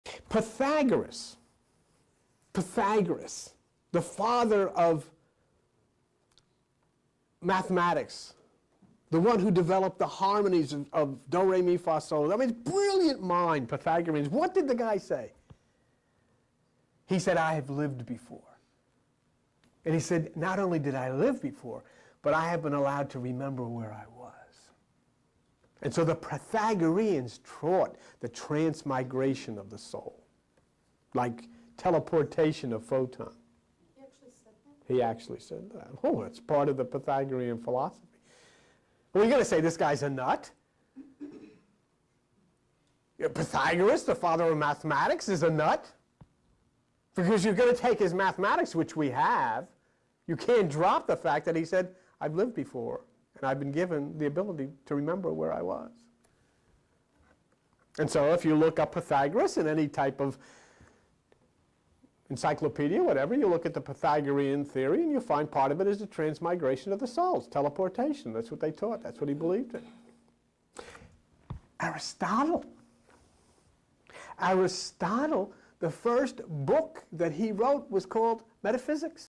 Pythagoras: The Father of Mathematics & Reincarnation
The father of mathematics, Pythagoras, believed in transmigration of the soul—teleportation, even. He claimed he'd lived before and could remember it. What does this say about the Pythagorean philosophy?